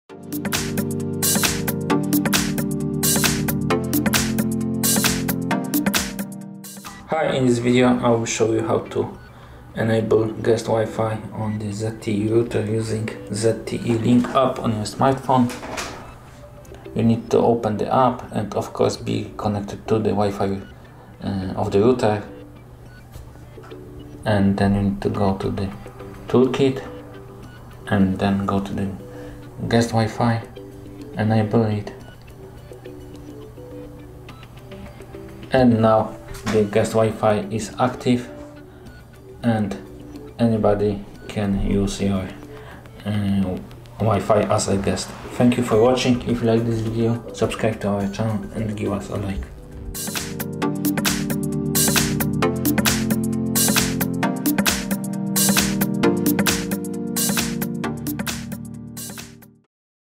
How to Enable Guest WiFi on ZTE MF283v Router - Create Separate WiFi Network for Clients and Guests
Watch this video to see the best and the easiest method of creating a Guest Wi-Fi network on the ZTE MF283v Router. This will greatly improve security because you will don't allow random people and devices to connect to your main WiFi network.
How to create a guest WiFi network on ZTE MF283v Router? How to find Guest  Network in ZTELink smartphone app?